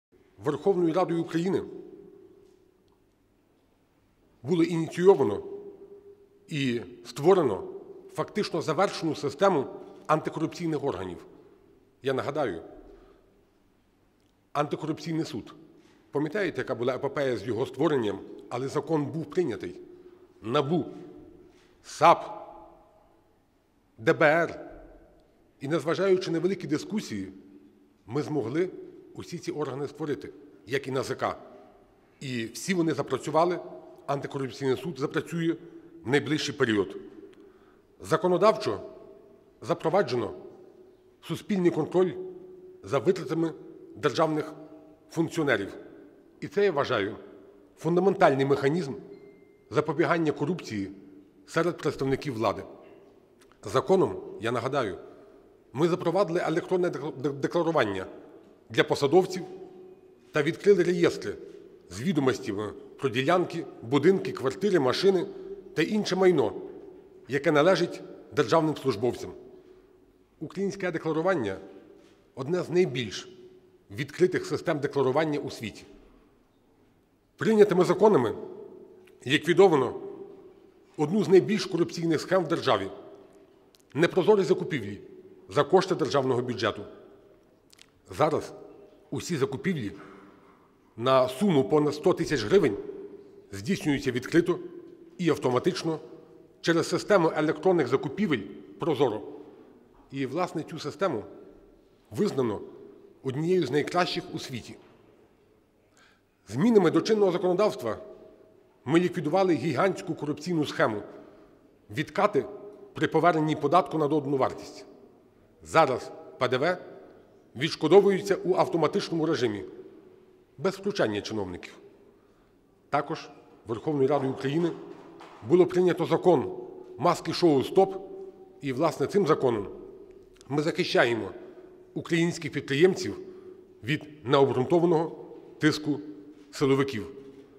Верховна Рада нинішнього скликання зробила титанічну роботу для протидії корупції, - Андрій Парубій
Верховна Рада восьмого скликання за свою каденцію здійснила ключові кроки для протидії корупції.

Про це заявив Голова Верховної Ради, представник «Народного фронту» Андрій Парубій на підсумковій прес-конференції в парламенті.

«ВР було ініційовано і практично завершено систему антикорупційних органів. Я нагадаю, Антикорупційний суд - закон був прийнятий. НАБУ, САП, ДБР - ми це створили. Антикорупційний суд запрацює в найближчий період», - повідомив спікер парламенту.

«Законодавчо запроваджений контроль за витратами державних функціонерів, і це, я вважаю, фундаментальний механізм протидії корупції в органах влади. Законом ми запровадили електронне декларування та запустили електронні реєстри про земельні ділянки, квартири, машини та інше майно, що належить державним посадовцям», - сказав Парубій.

Голова Верховної Ради також згадав і про систему RroZorro, яка визнана одною з кращих у світі.

«Ми ліквідували систему гігантських відкатів при поверненні ПДВ, зараз воно повертається в електронному режимі, без втручання чиновників. Також ВРУ було ухвалено закон «маски-шоу стоп», і цим ми запобігаємо втручанню силовиків у бізнес», -  підкреслив Парубій.

Приєднуйся!